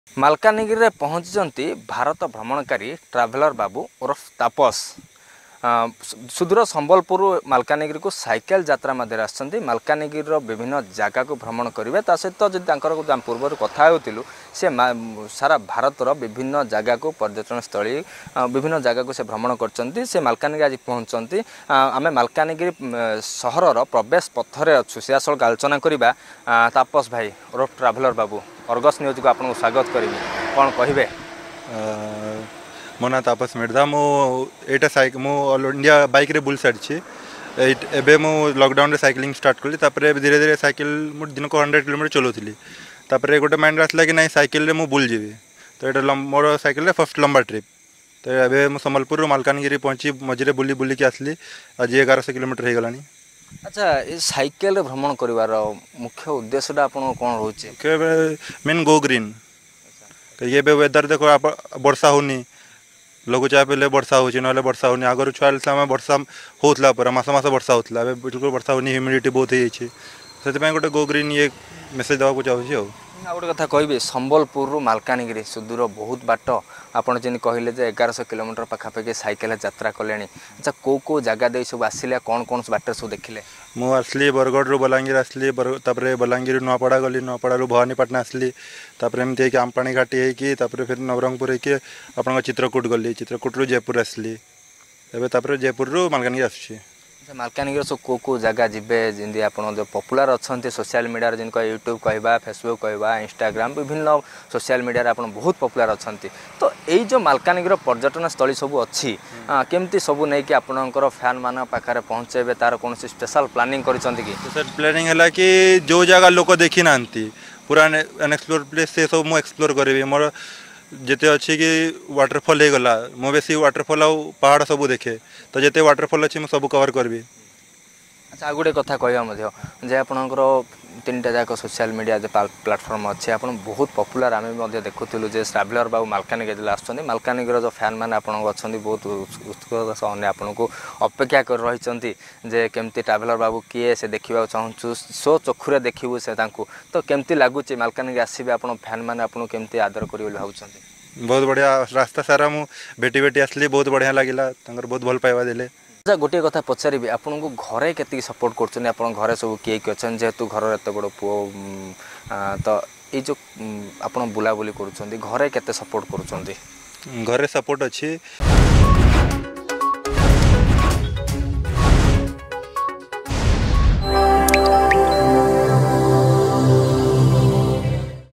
ମାଲକାନଗିରି ଜିଲ୍ଲାରେ ଭାରତ ଭ୍ରମଣକାରୀ ଟ୍ରାଭେଲର ବାବୁ ତାପସ ମ୍ରିଧା
ମାଲକାନଗିରି ଜିଲ୍ଲାରେ ଭାରତ ଭ୍ରମଣକାରୀ ଟ୍ରାଭେଲର ବାବୁ ତାପସ ମ୍ରିଧା । ପୂର୍ବରୁ ଦେଶର ବିଭିନ୍ନ କୋଣ ଅନୁକୋଣ ସାଇକେଲ ଓ ମଟର ସାଇକେଲରେ ଭ୍ରମଣ କରିସାରିଥିବା ବେଳେ ଏବେ ସେ ସାଇକେଲରେ ମାଲକାନଗିରି ଭ୍ରମଣ କରିବାକୁ ଆସିଛନ୍ତି । ସୁଦୂର ସମ୍ବଲପୁରରୁ ମାଲକାନଗିରି ସାଇକେଲ ମାଧ୍ୟମରେ ଯାତ୍ରା ଆରମ୍ଭ କରିଥିବା ବେଳେ ଦୀର୍ଘ 1100 କିଲୋମିଟର ସାଇକେଲ ଚଳାଇଲେଣି ।